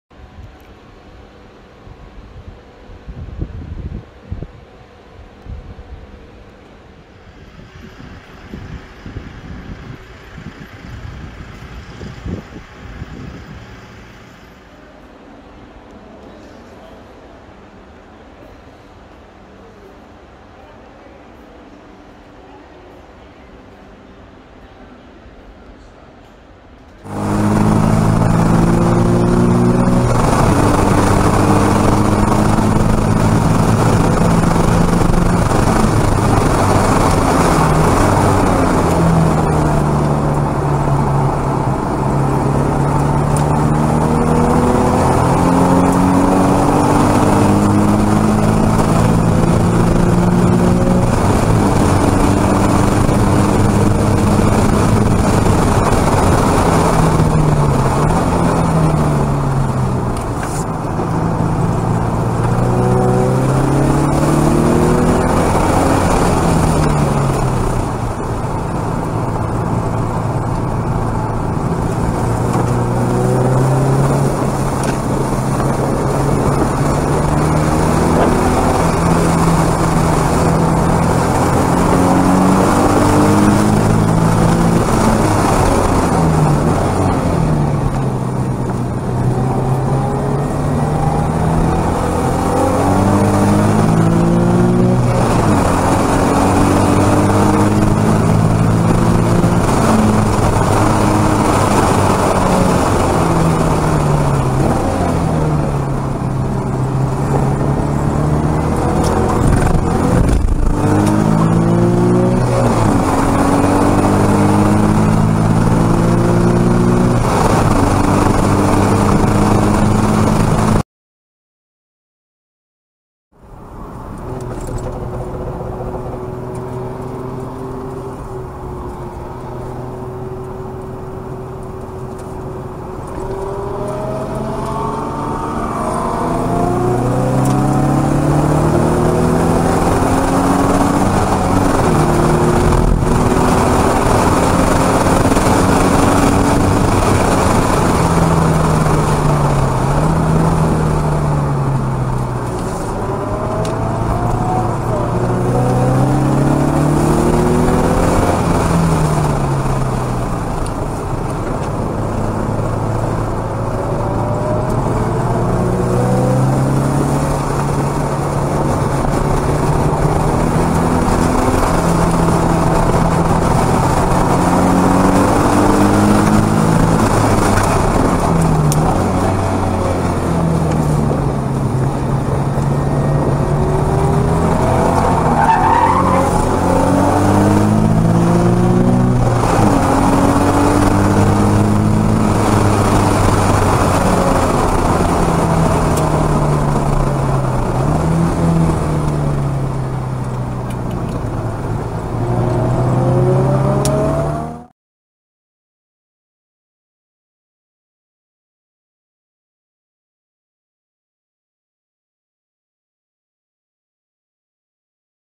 the 350z Back to track - Bahrain inner Circuit track day
19 Dec 2020
Best lap 1:27
some old Achilies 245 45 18 tires
Weather is awesome ..